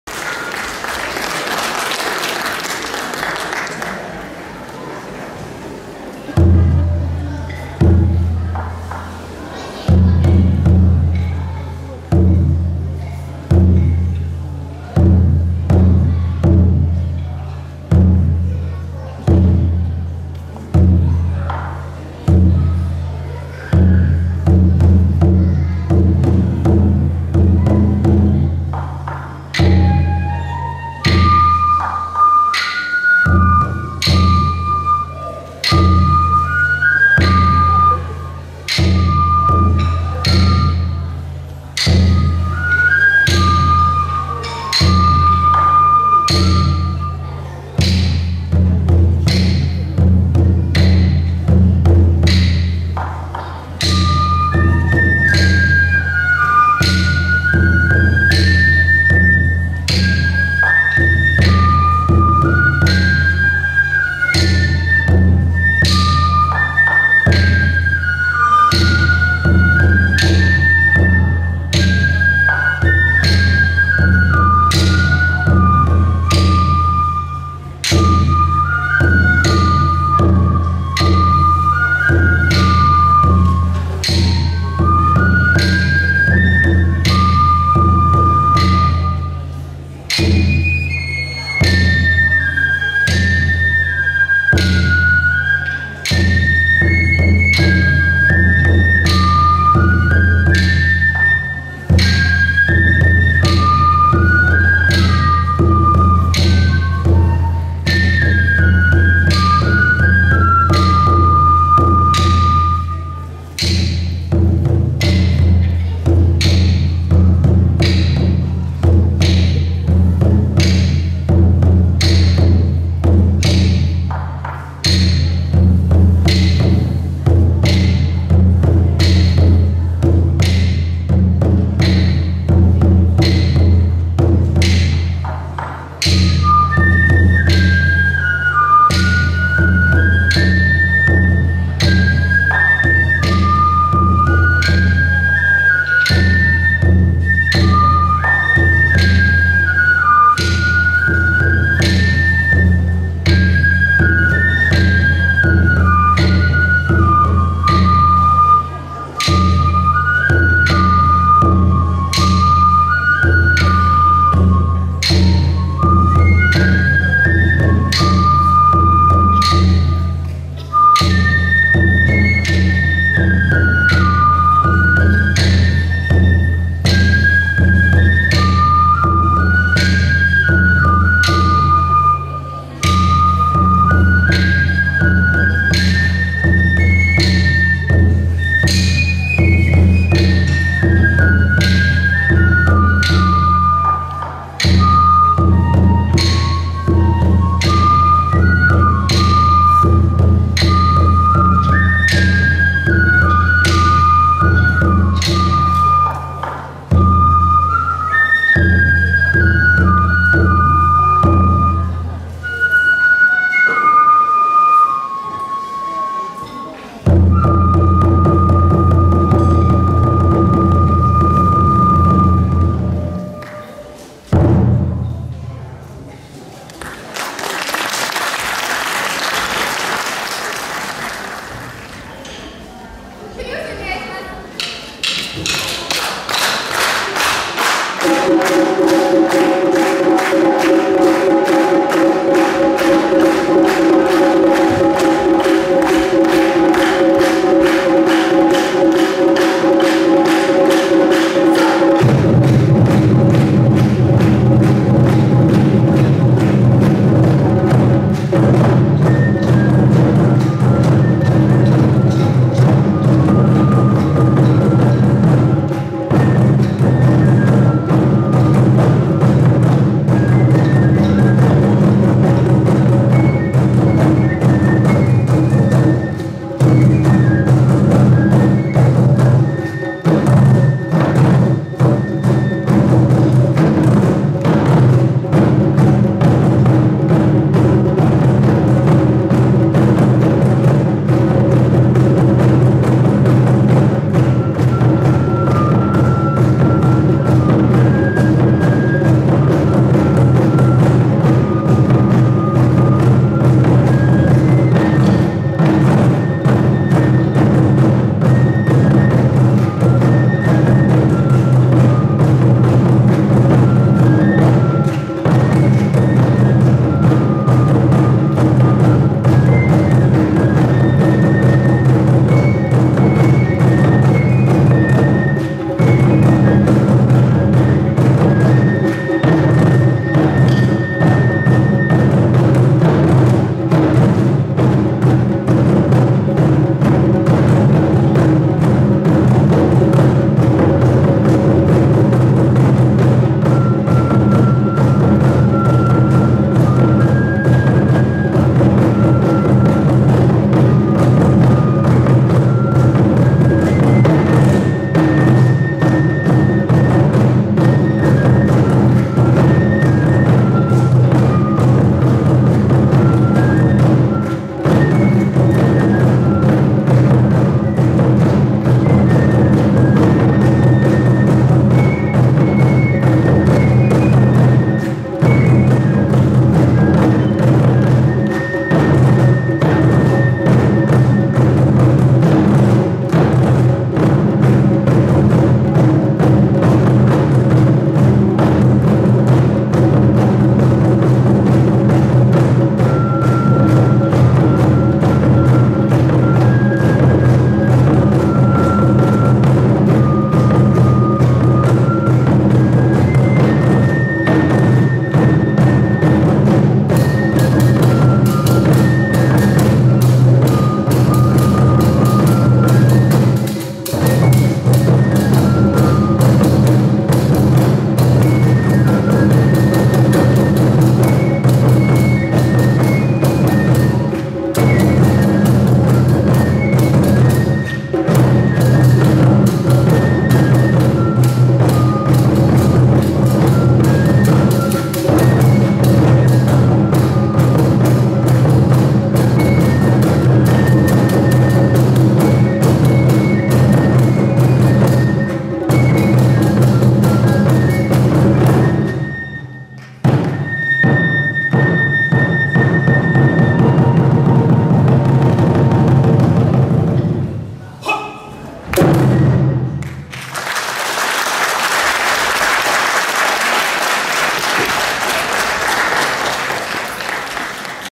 総領響心太鼓
第13回TAIKO交流会
平成26年11月30日（日）
高小学校体育館
広島県庄原市の和太鼓団体が一堂に会する演奏会です。